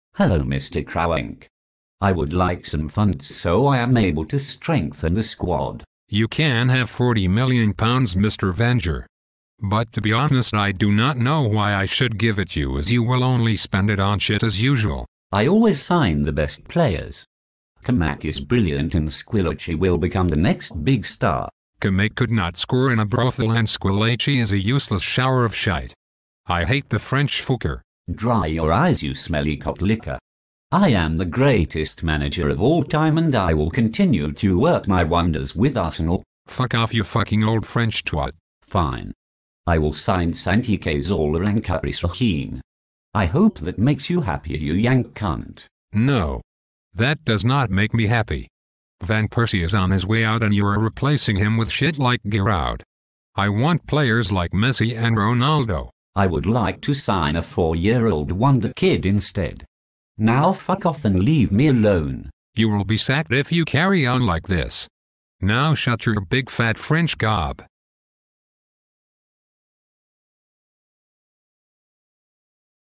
Arsene Wenger and Stan Kroenke pre-season meeting
Arsene Wenger has a pre-season meeting with Stan Kroenke.